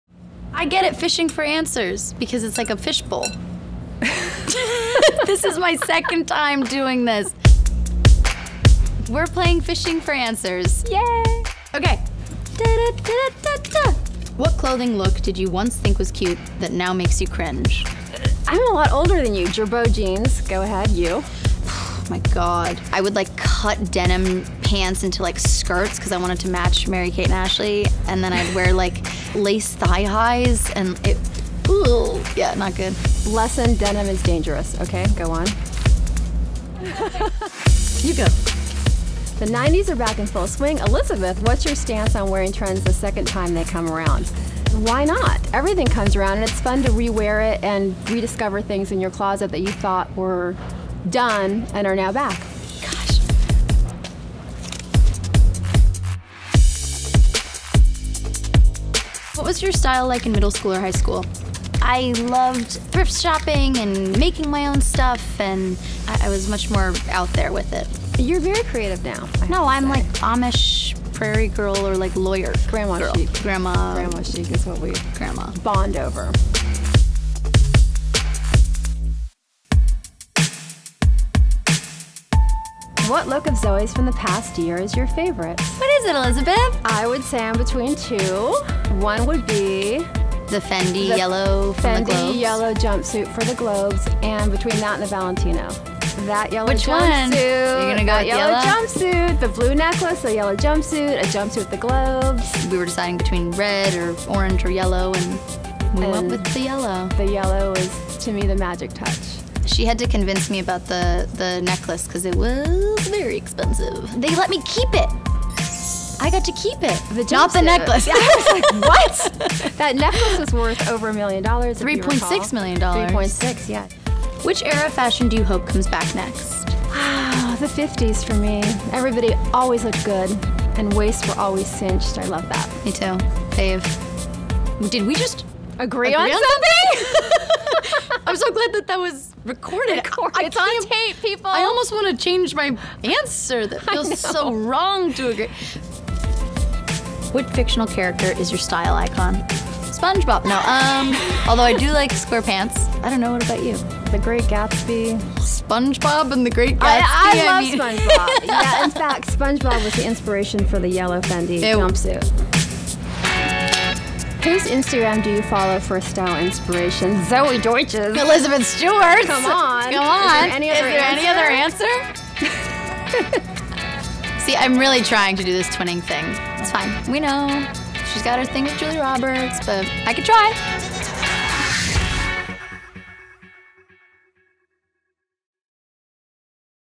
'The Politician' Star Zoey Deutch & Stylist Elizabeth Stewart Play 'Fishing for Answers' | THR
Zoey Deutch ('The Politician') shared her favorite looks and her personal style inspirations with her stylist Elizabeth Stewart for the 2020 Power Stylists issue.